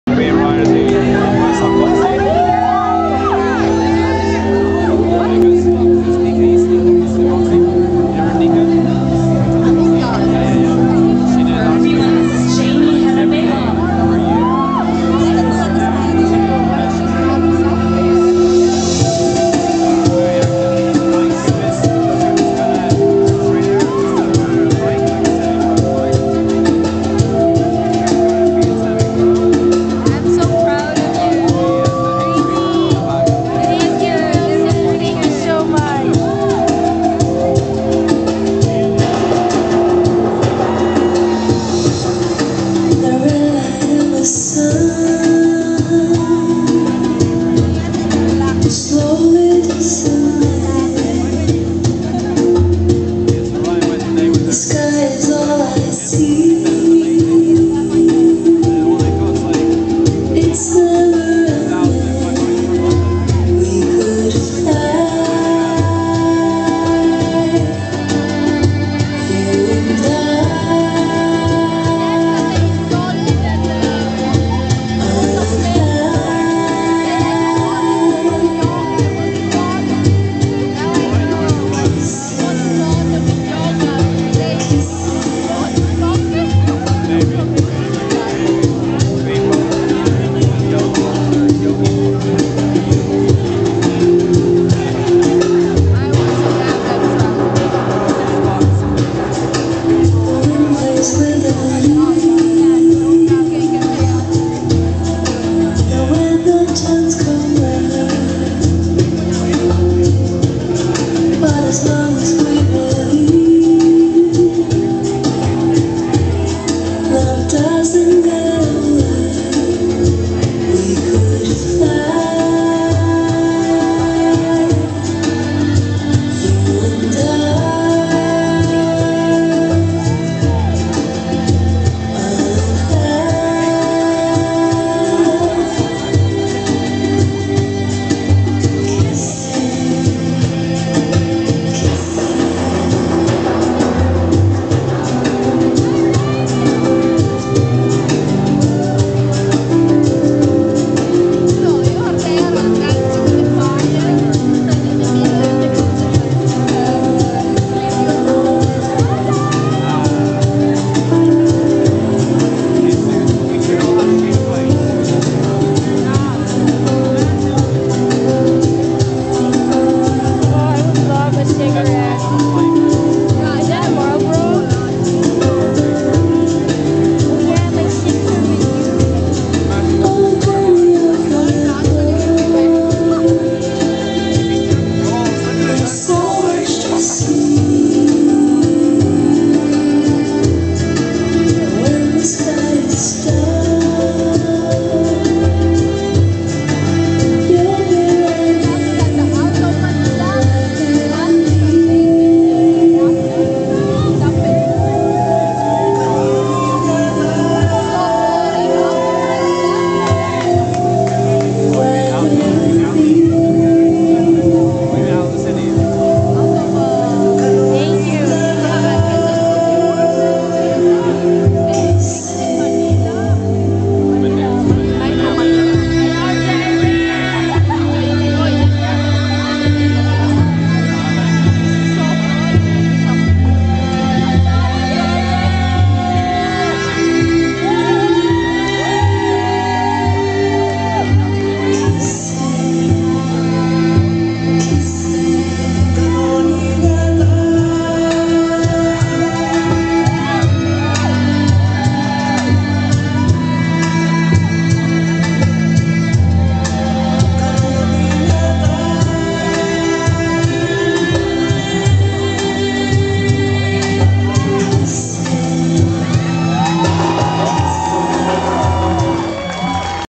Jamie Jarabejo on Fire Pois @ SpinD_11.24.12
Jamie Jarabejo enchants the crowd at Saturday's Spindependence gig at Arts in the City.